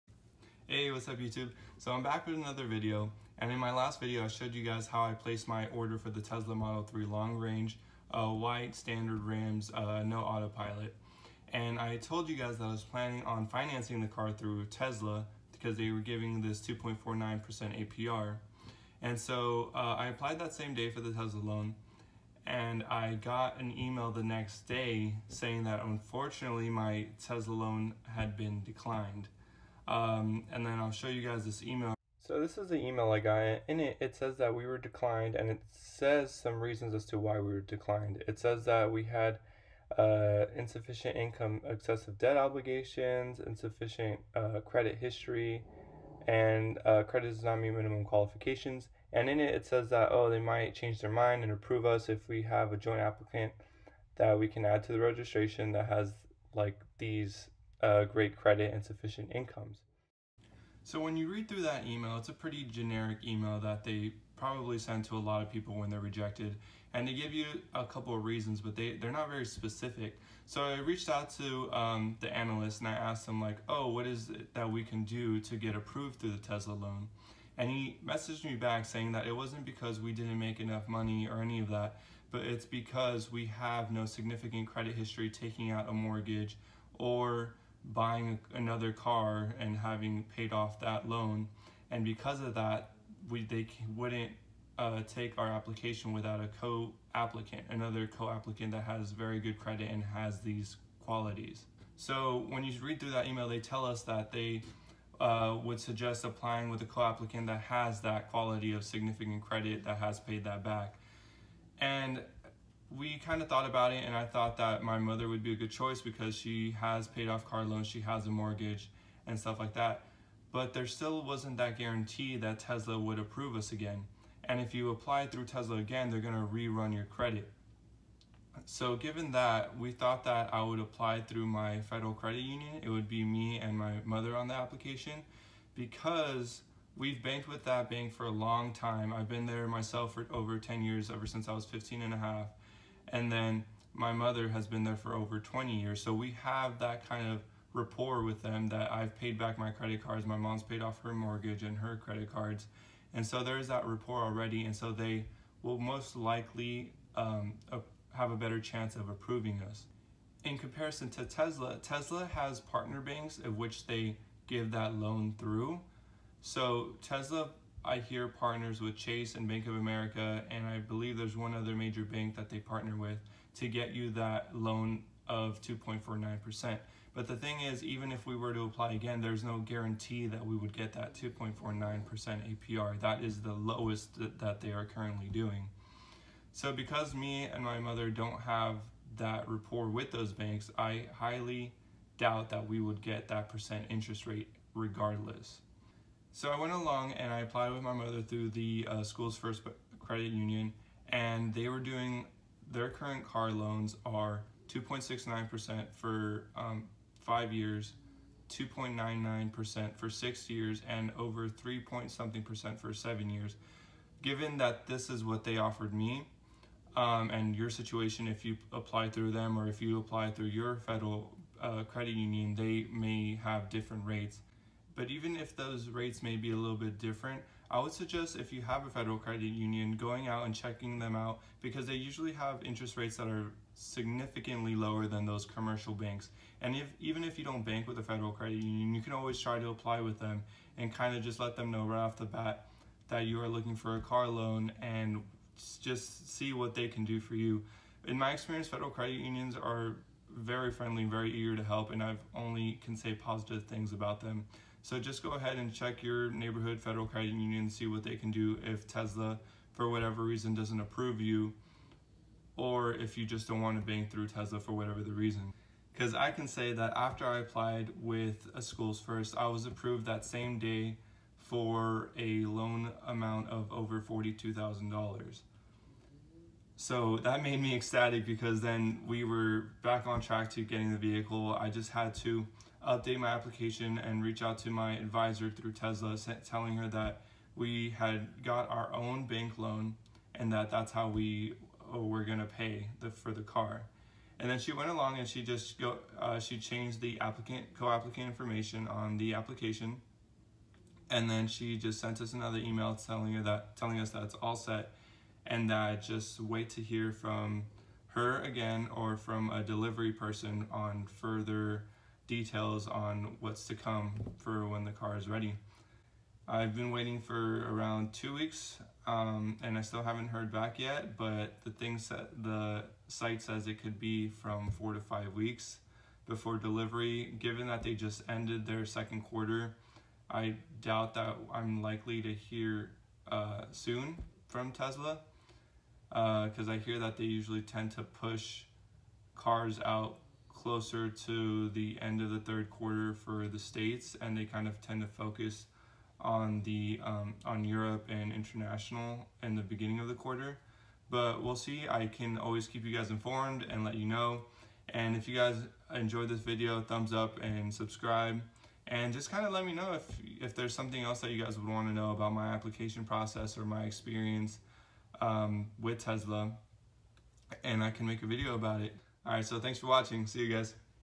Tesla DENIED financing! Here`s what I did to get approved for my Model 3.
Recently put in an order for a Tesla Model 3 Long Range. Couple days letter I was denied financing through Tesla. Here is what I did to get financing approved.